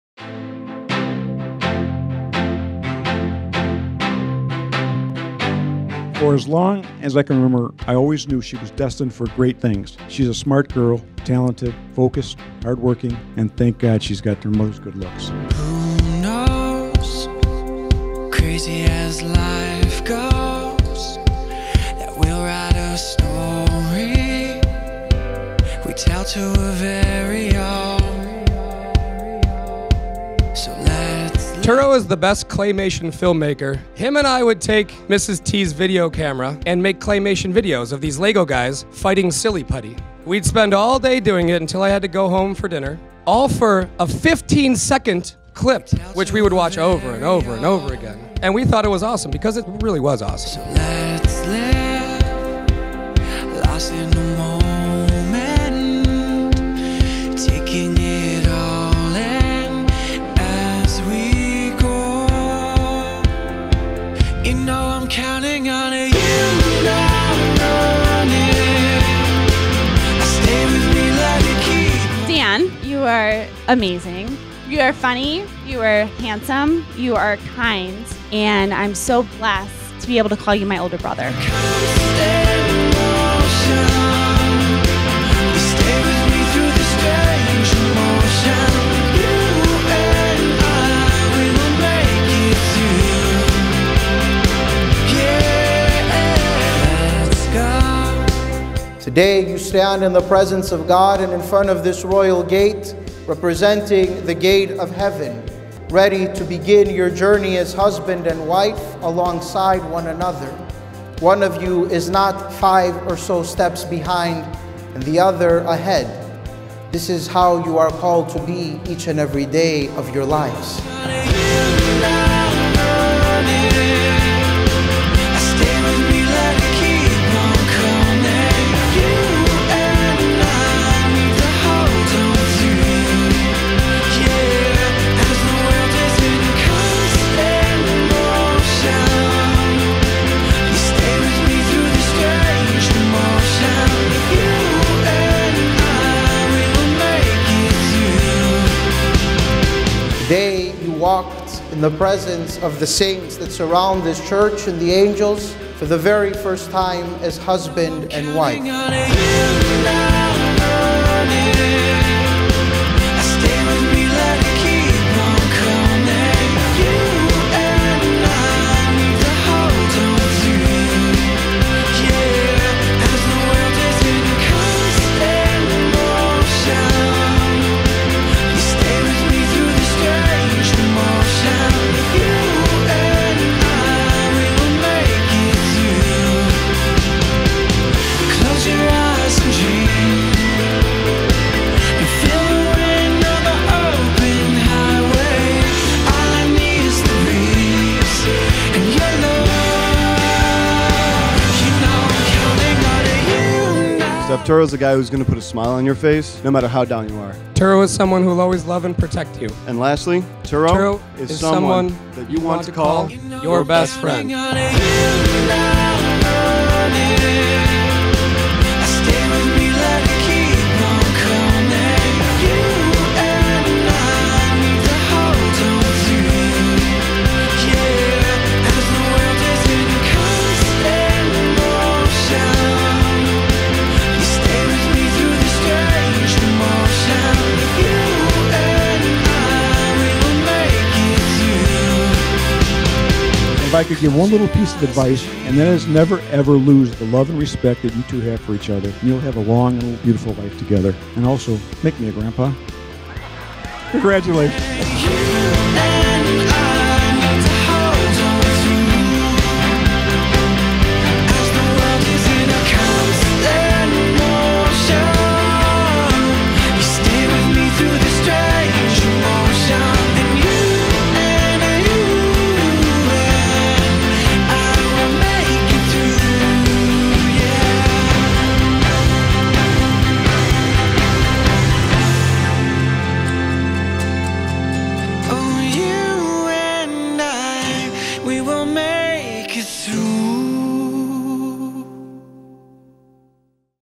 Steph and Dan's Wedding Trailer
Dan’s proposal to Steph involved a city wide scavenger hunt eventually leading Steph to Dan in a park.  Their wedding at the Metropolitan Club atop the Willis Tower was a perfect place to host their closest family and friends.  The waterfall of shots and jump rope dance were pretty fun to witness too!

VENUE  @annunciation_cathedral_chicago
PHOTO @mikestaffproductions
VIDEO @smilingtoadproductions
FLORAL by Analyn Dare
MUSIC @mikestaffproductions
TRANSPORT @windycitylimo